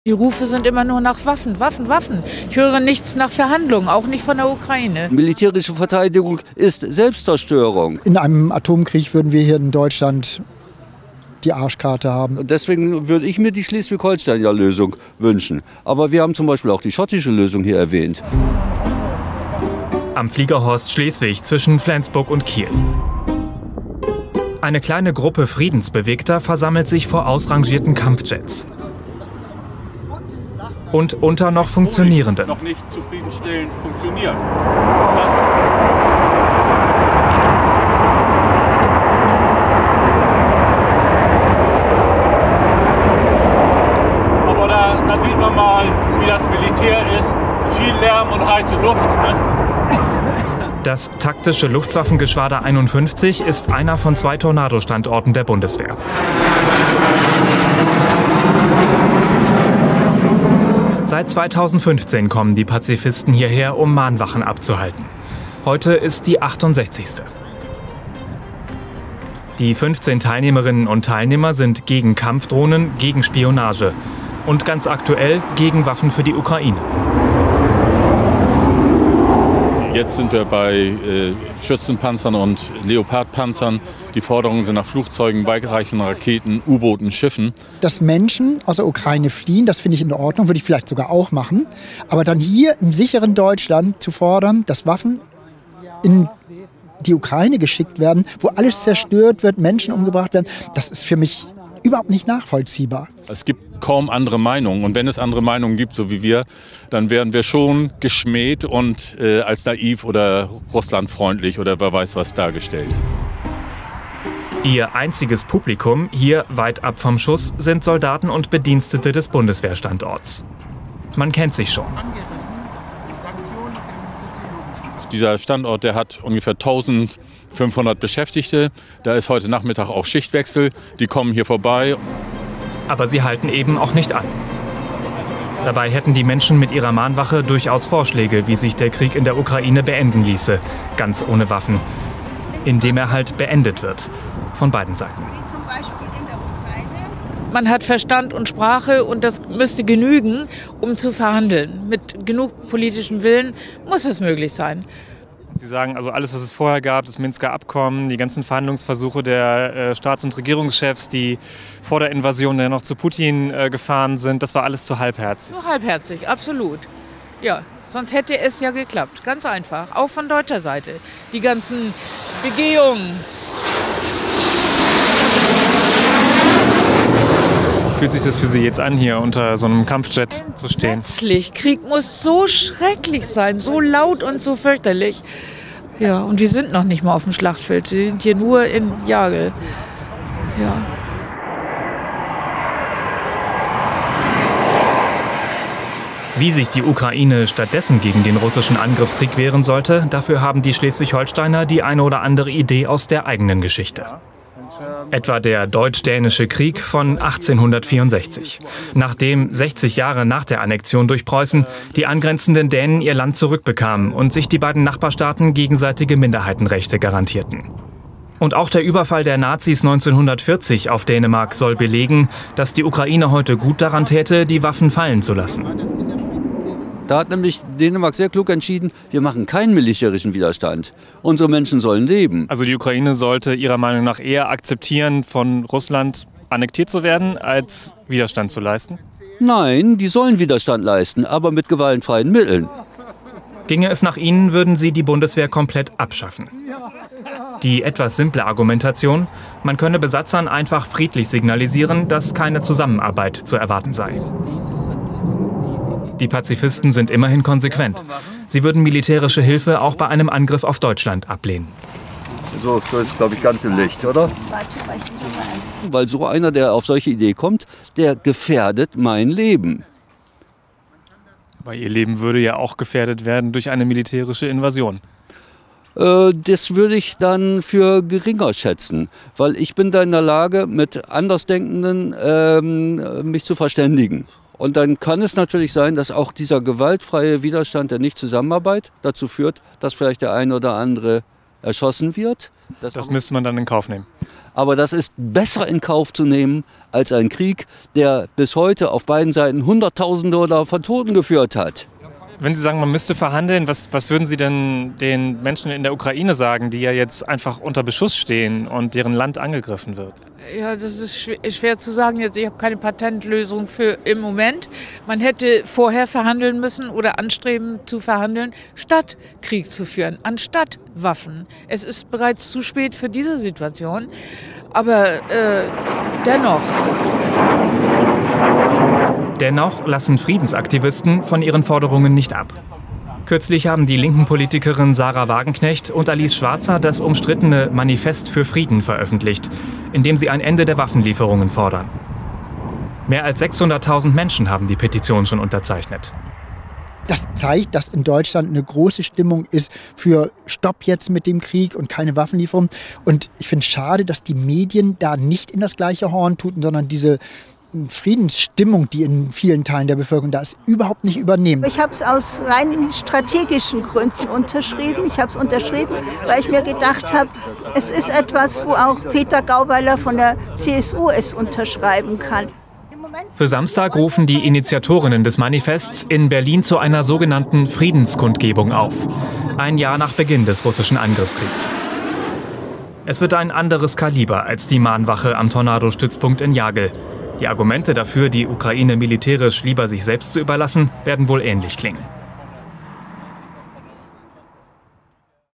Schwarzer-Wagenknecht-Manifest: Geschichtsverklärung am Jagelzaun | DER SPIEGEL
Friedensaktivisten halten – zum 68. Mal – am Tornadostützpunkt in Schleswig-Holstein eine Mahnwache gegen Waffen. Die kleine Gruppe fordert ein Ende des Kriegs. Die Ukraine könne sich ja friedlich wehren.